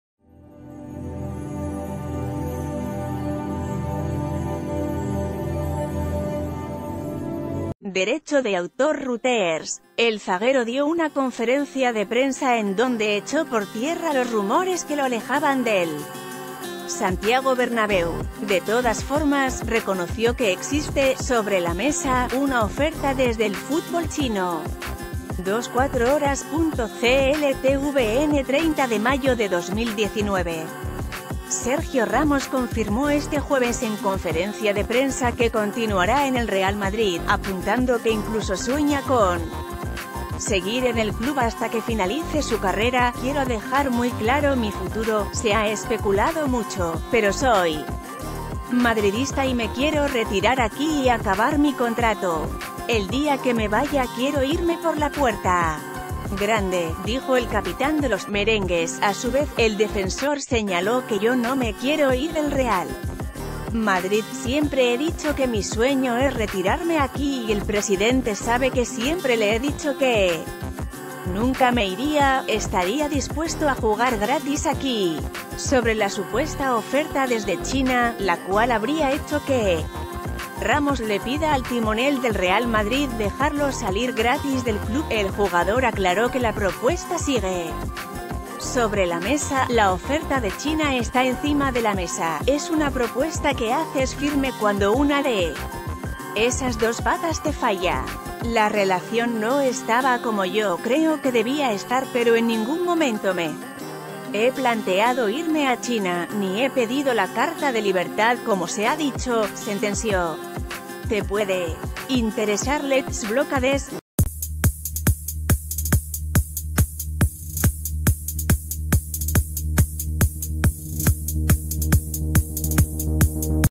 Sergio Ramos anunció que continuará en el Real Madrid: "Mi sueño es retirarme aquí"
El zaguero dio una conferencia de prensa en donde echó por tierra los rumores que lo alejaban del Santiago Bernabéu. De todas formas, reconoció que existe "sobre la mesa" una oferta desde e...